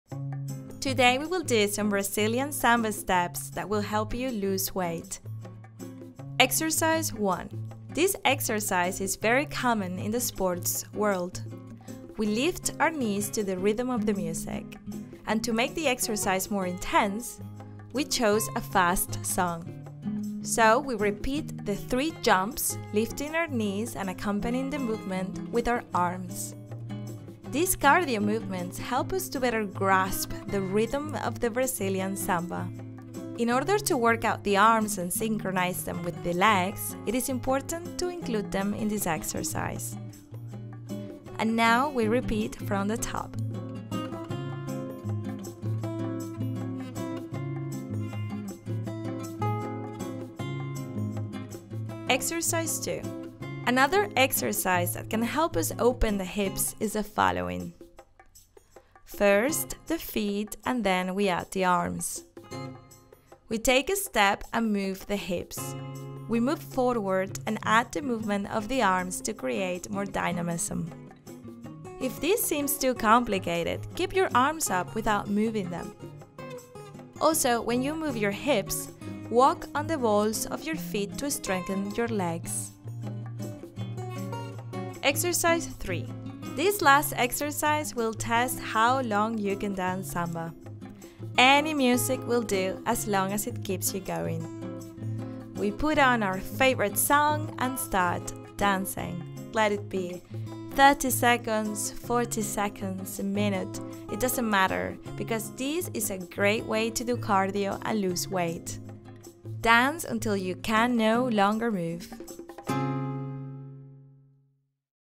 Dance SAMBA to LOSE WEIGHT ✨ (Exercises and benefits)
In this oneHOWTO video we see how to dance samba to lose weight. This samba course for beginners explains the benefits of dancing samba and the exercises you can do to dance samba and lose weight. Learn how to dance samba to lose weight and do dance exercises for samba dancing.

0:00| start
0:14| benefits of samba
0:51| exercise 1
2:05| exercise 2
3:46| exercise 3
5:38| other videos